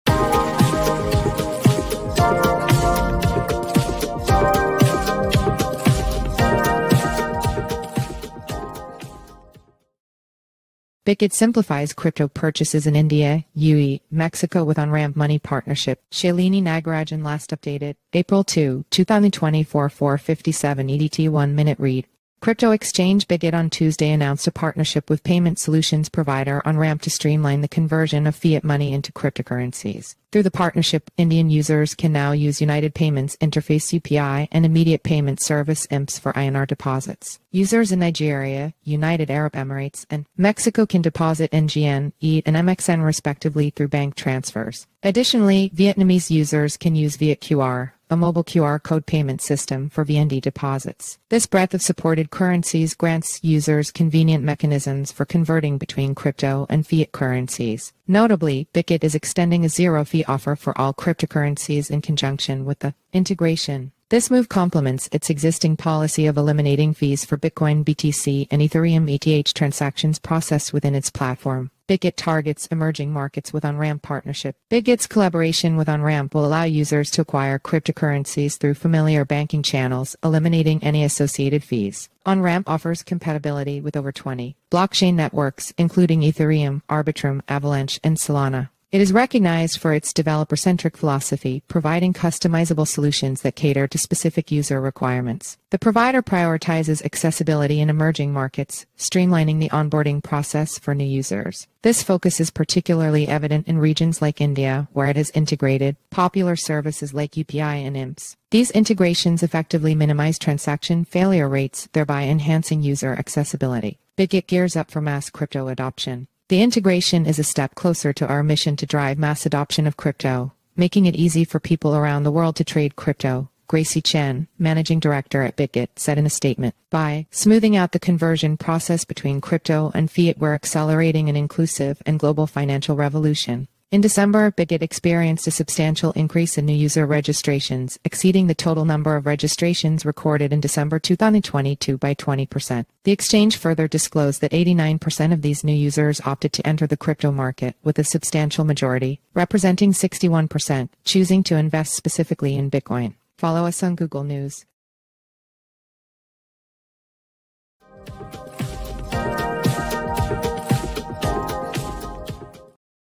Bitget Simplifies Crypto Purchases in India, UAE, Mexico with OnRamp
Shalini Nagarajan

Last updated:

April 2, 2024 04:57 EDT

Crypto exchange Bitget on Tuesday announced a partnership with payment solutions provider OnRamp to streamline the conversion of fiat money into cryptocurrencies.

Through the partnership, Indian users can now use United Payments Interface (UPI) and Immediate Payment Service (IMPS) for INR deposits. Users in Nigeria, United Arab Emirates, and Mexico can deposit NGN, AED, and MXN respectively through bank transfers.

Additionally, Vietnamese users can use VietQR, a mobile QR code payment system, for VND deposits.

This breadth of supported currencies grants users convenient mechanisms for converting between crypto and fiat currencies.

Notably, Bitget is extending a zero-fee offer for all cryptocurrencies in conjunction with the integration. This move complements its existing policy of eliminating fees for Bitcoin (BTC) and Ethereum (ETH) transactions processed within its platform.

Bitget Targets Emerging Markets with OnRamp Partnership.

Bitget’s collaboration with OnRamp will allow users to acquire cryptocurrencies through familiar banking channels, eliminating any associated fees.

OnRamp offers compatibility with over 20 blockchain networks, including Ethereum, Arbitrum, Avalanche, and Solana. It is recognized for its developer-centric philosophy, providing customizable solutions that cater to specific user requirements.

The provider prioritizes accessibility in emerging markets, streamlining the onboarding process for new users. This focus is particularly evident in regions like India, where it has integrated popular services like UPI and IMPS. These integrations effectively minimize transaction failure rates, thereby enhancing user accessibility.

Bitget Gears Up for Mass Crypto Adoption.

“The integration is a step closer to our mission to drive mass adoption of crypto, making it easy for people around the world to trade crypto,” Gracy Chen, Managing Director at Bitget, said in a statement. “By smoothing out the conversion process between crypto and fiat we’re accelerating an inclusive and global financial revolution.”

In December, Bitget experienced a substantial increase in new user registrations, exceeding the total number of registrations recorded in Dec. 2022 by 20%. The exchange further...